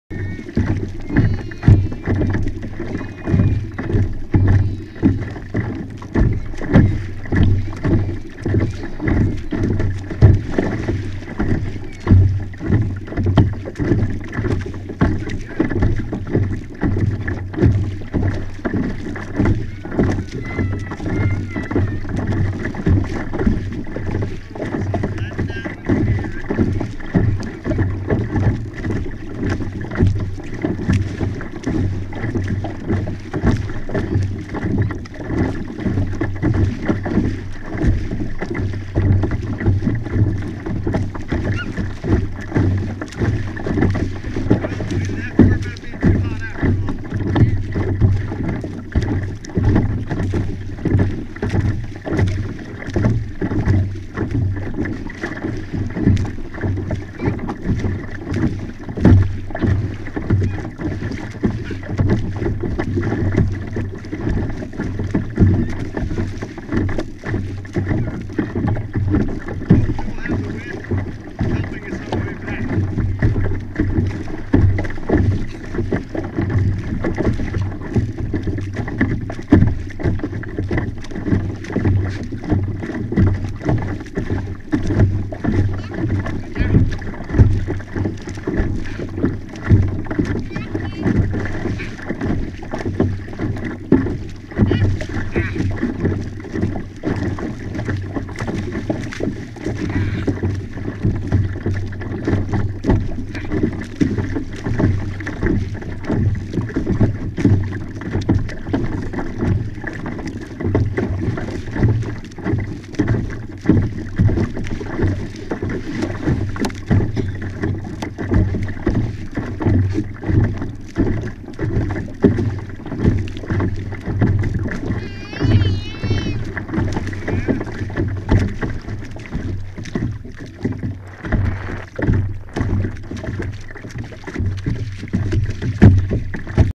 Zoe's first kayak adventure, truckin along with Dada
Footage using Drift HD camera in Marina Del Ray, with Hobie Outback pedal drive kayaks.